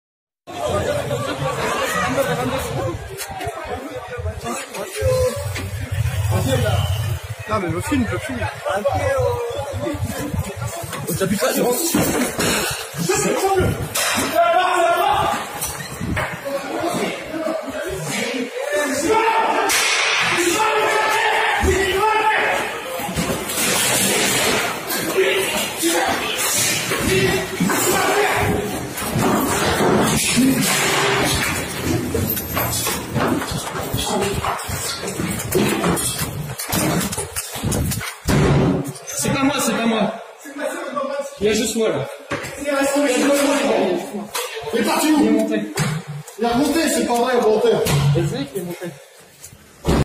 Amadou agressé par 4 policiers dans sa Cité U
Amadou, un étudiant de 23 ans, a été violemment frappé par des policiers au sein même de sa Résidence Universitaire de la Pitié-Salpêtrière lors de la manif du 1er mai. "L'intrusion violente", ce sont ces 4 membres des forces de l'ordre qui l'ont commise.

Vidéo à voir absolument, à LIKER et à PARTAGER !
Incroyable : 4 agents des forces de l’ordre 👮🏻‍♂️ tabassent et poursuivent AMADOU un étudiant à l'intérieur de sa résidence universitaire durant les manifestations des GiletsJaunes YellowVests du 1erMai!!
► Si vous avez aimé cette vidéo, n'oubliez pas de la liker, en cliquant sur le petit pouce 👍 sous la vidéo.